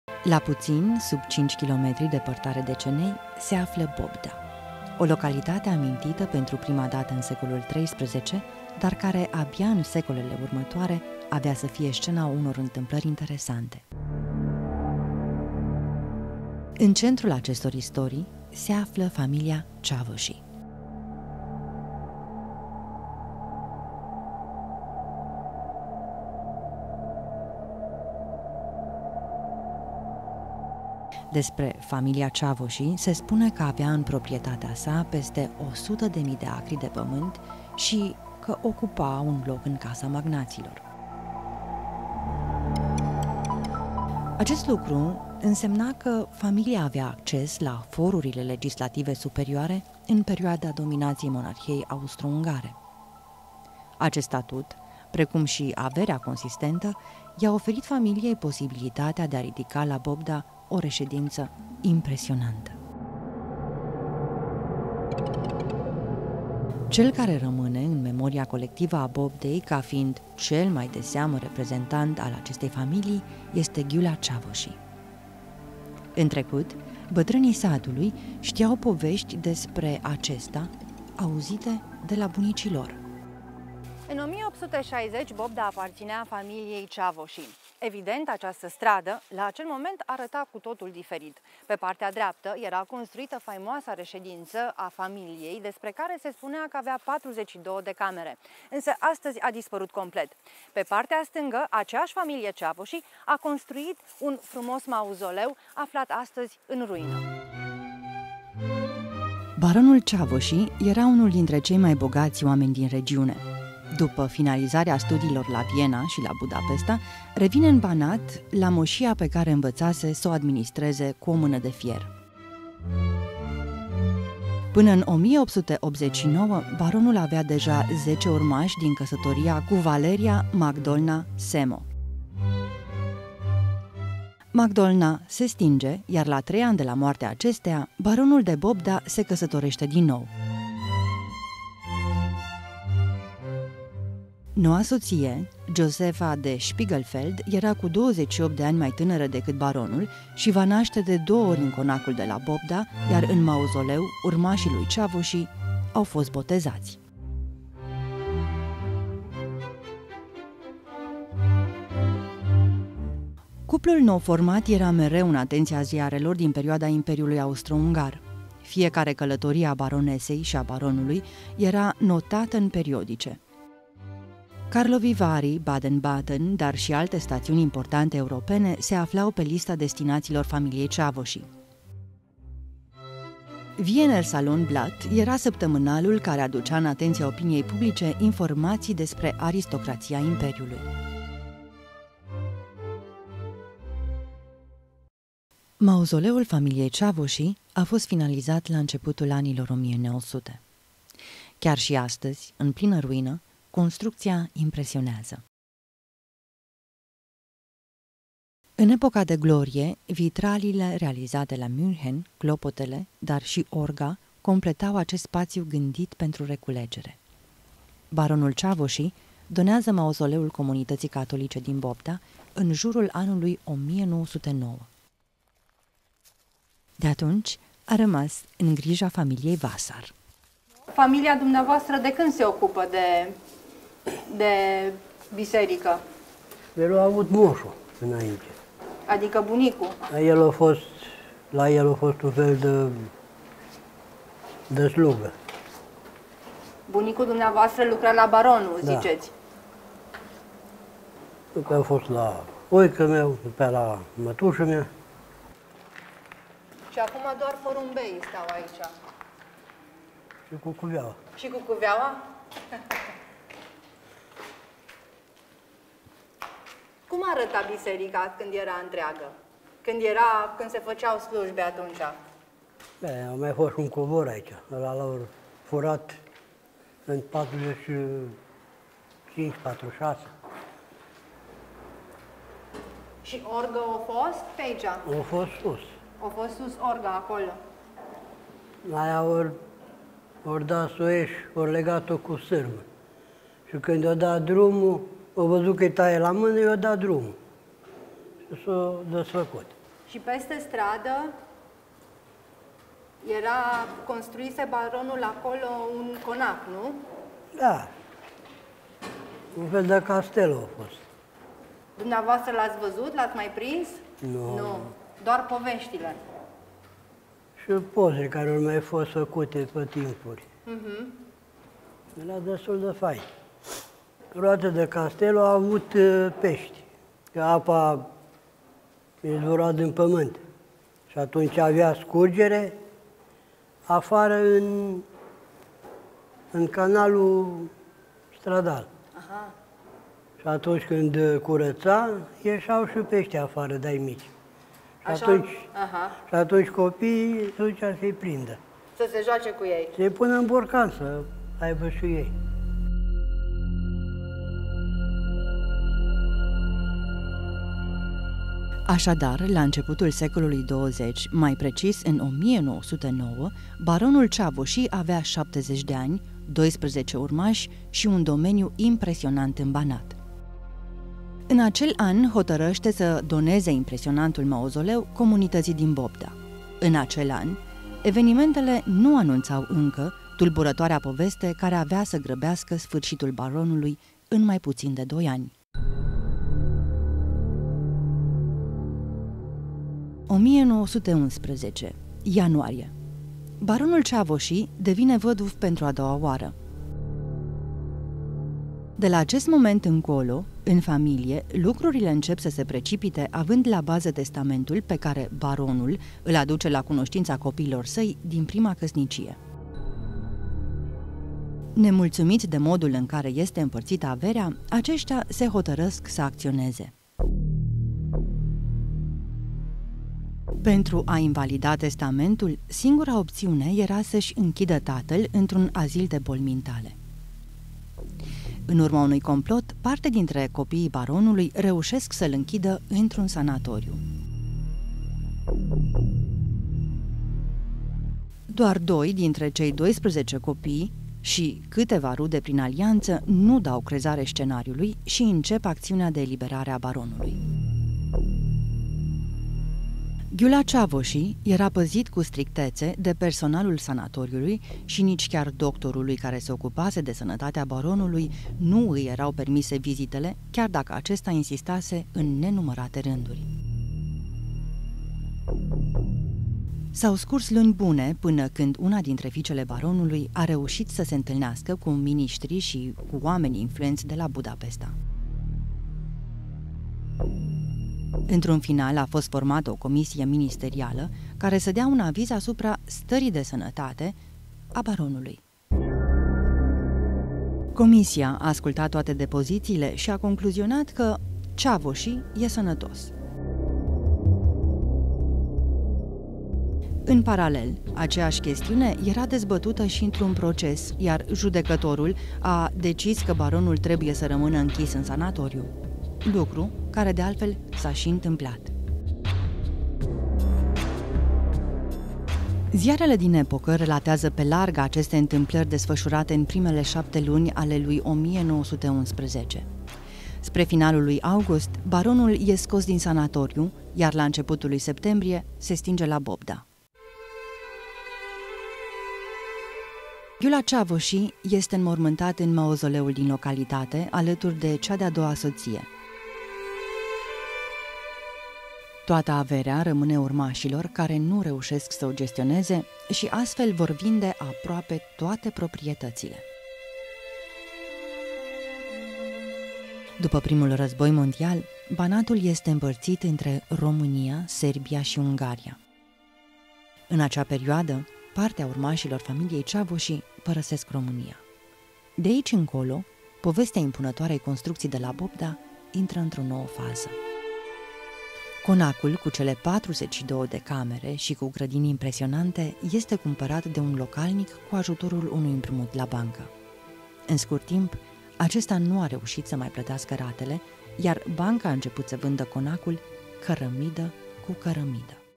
Mausoleul de la Bobda: Mărirea și decăderea baronului Csavossy
Despre familia Csvossy se spune că avea în proprietate peste 10000 de acri de pământ și ocupa un loc în casa Magnaților. Asta însemna că familia avea acces la forurile legislative superioare în perioada dominației Monarhiei austro-ungare. Acest statut precum și averea consistentă i-a oferit familiei posibiliatatea de a ridica la Bobda o reședință impresionantă. În același timp, a donat sătenilor o contrucție impunătoare.